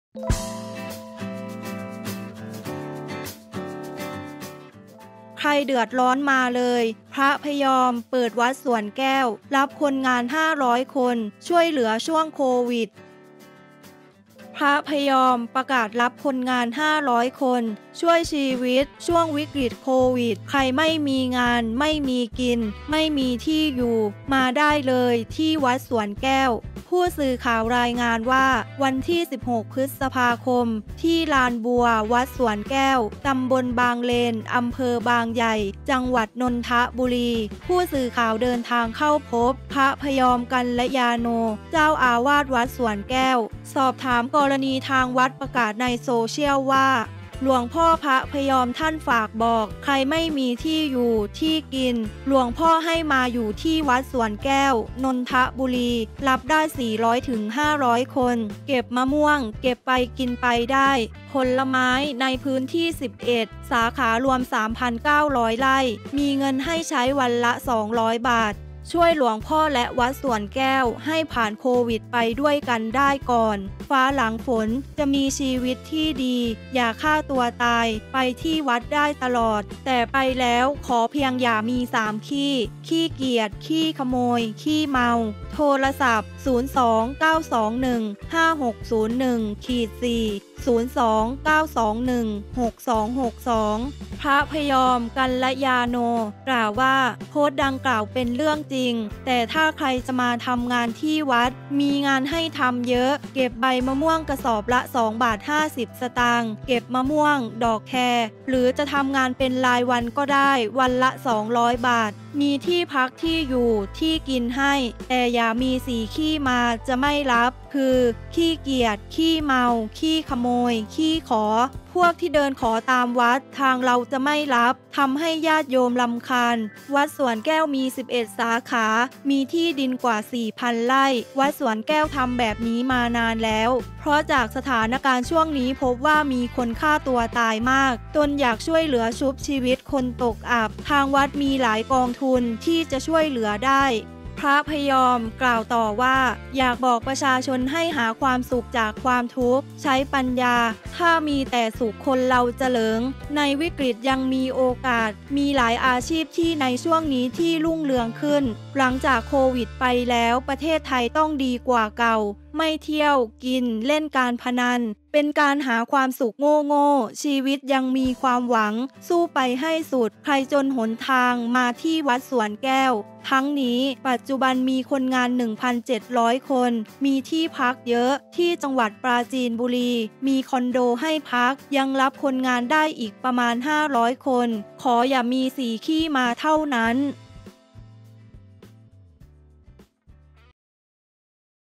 ใครเดือดร้อนมาได้เลย! พระพยอม เปิดวัดสวนแก้ว รับคนงาน 500 คน ช่วยเหลือช่วงโควิด
พระพยอม ประกาศรับคนงาน 500 คน ช่วยชีวิตช่วงวิกฤตโควิด ใครไม่มีงาน ไม่มีกิน ไม่มีที่อยู่ มาได้เลยที่วัดสวนแก้ว

ผู้สื่อข่าวรายงานว่า วันนี้ (16 พ.ค.) ที่ลานบัว วันสวนแก้ว ต.บางเลน อ.บางใหญ่ จ.นนทบุรี ผู้สื่อข่าวเดินทางเข้าพบ พระราชธรรมนิเทศ (พยอม กัลยาโณ) เจ้าอาวาสวัดสวนแก้ว สอบถามกรณีที่ทางวัดประกาศในโซเชียลว่า

"หลวงพ่อพระพยอมท่านฝากบอก ใครไม่มีที่อยู่ ที่กิน หลวงพ่อให้มาอยู่ที่​วัดสวนแก้ว นนทบุรี​รับได้ 400-500 คน เก็บมะม่วง เก็บไปกินไปก็ได้ ​(ผลไม้ ฯลฯ ในพื้นที่ 11 สาขารวม 3,900 ไร่)​ มีเงินให้ใช้วันละ 200 บาท